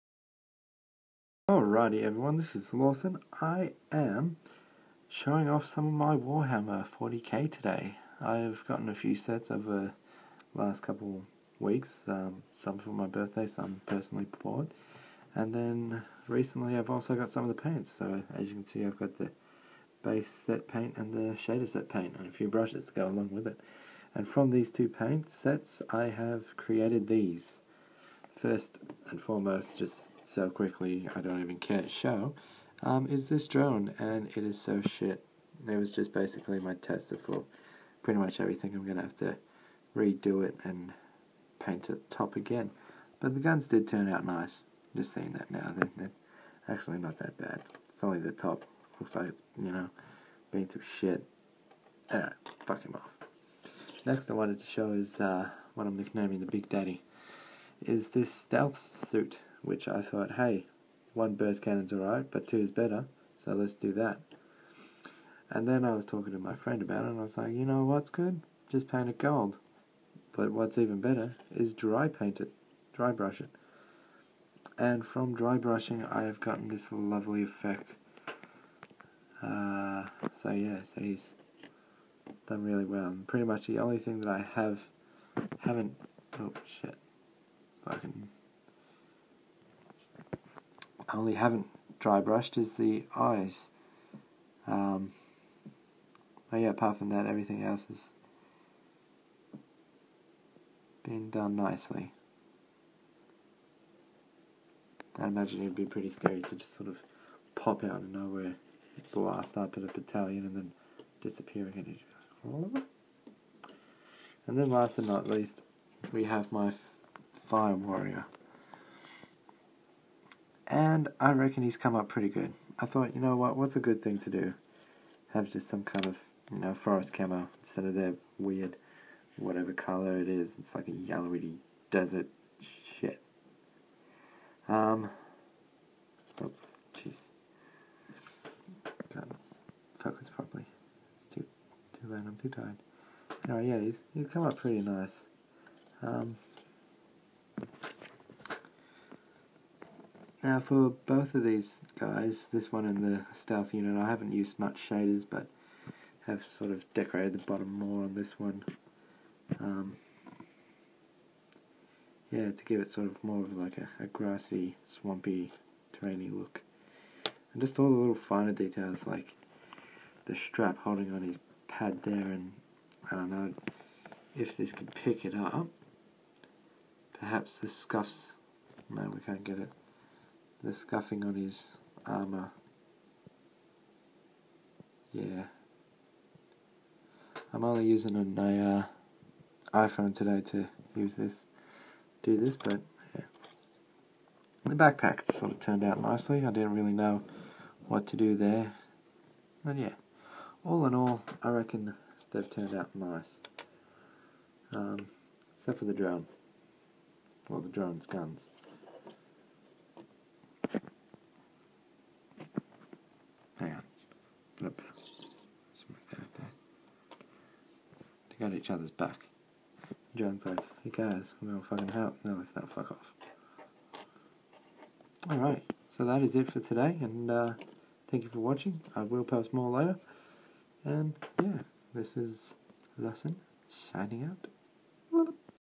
Lawson shows off Tau 40K 1
Just a quick heads up sorry for the blurriness in this video, but hopefully i haven't done that bad of a job on my first paint jobs.